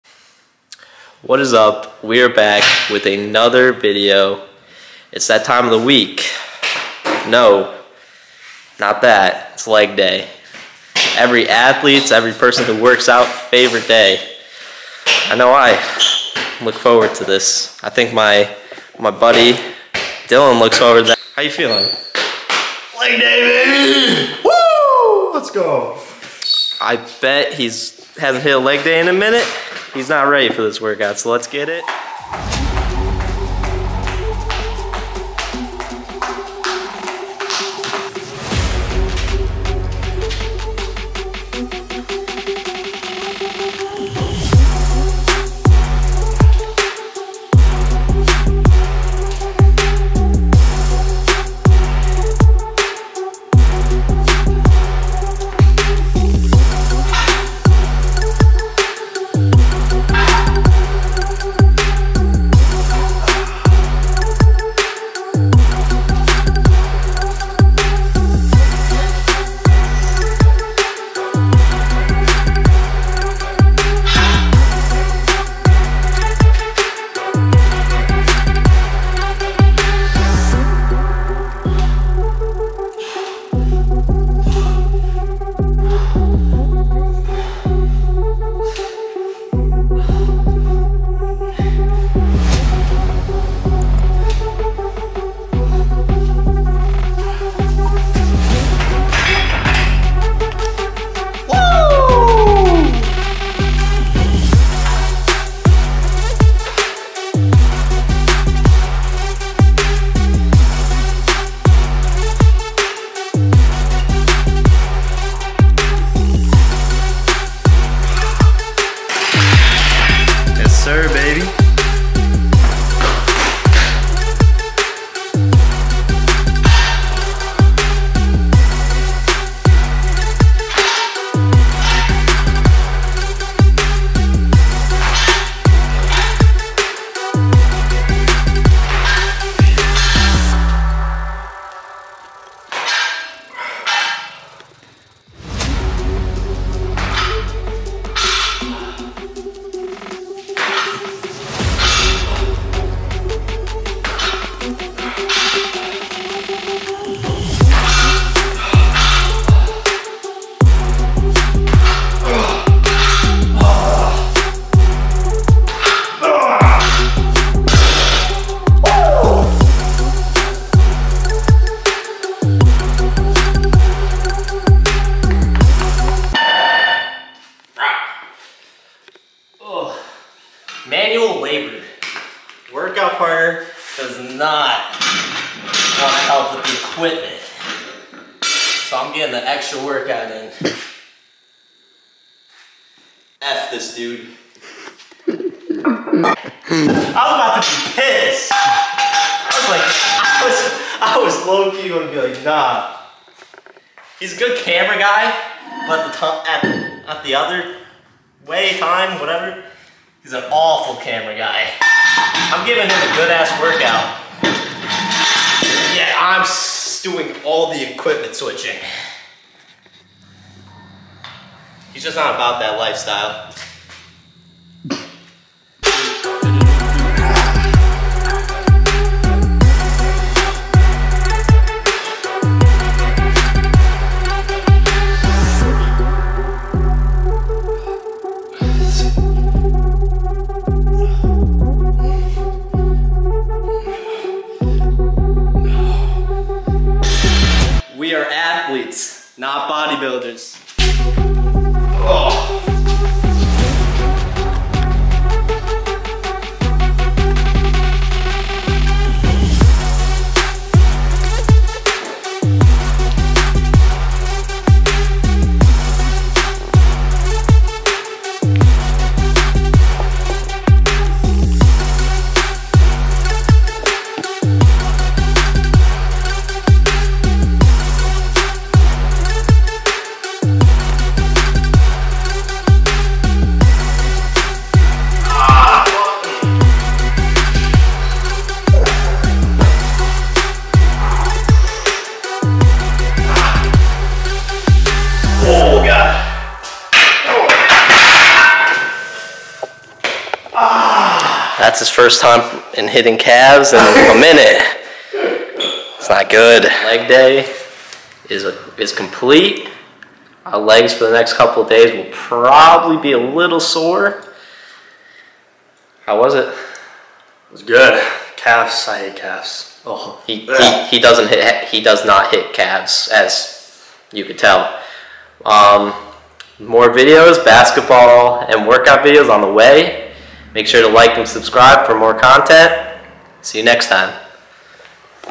Become more EXPLOSIVE Full Leg Day Workout | Gym Vlog #2
Athletic Leg Day Workout that helps build strength but also explosiveness, this video is for athletes that want to take their game to the next level, make sure to follow my socials for more workouts.